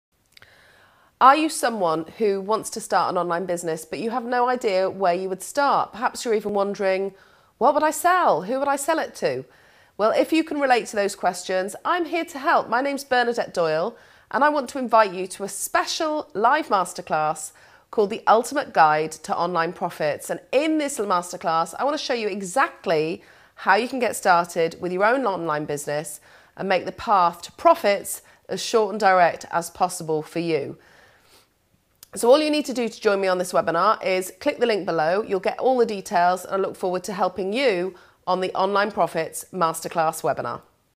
How to start an online business
Would you like to start an online business? Are you wondering how to start an online business?

In my 15 years of running my own online business, I know where to start and how to help you navigate the important areas.

Register for my Free Live Masterclass and I will explain all.